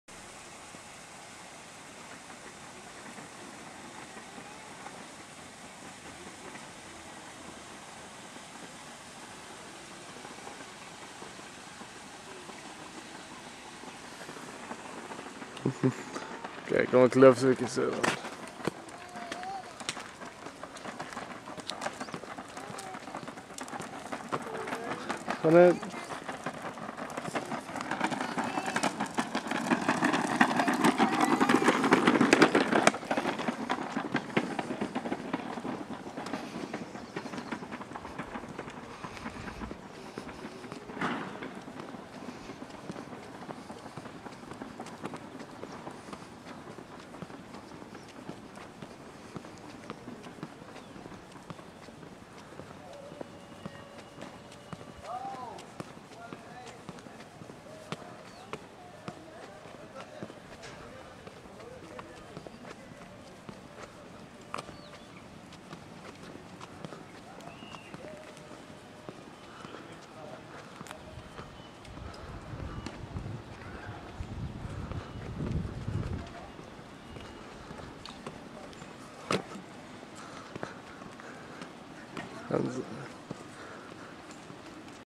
Pine Lake Marina Reception to Piazza.MOD
Pine Lake Marina is situated in the Garden Route - South Africa, near the town of Sedgefield. This is a walk from the reception area to the Piazza, where there is a shop, coffee bar and gym. or phone 0027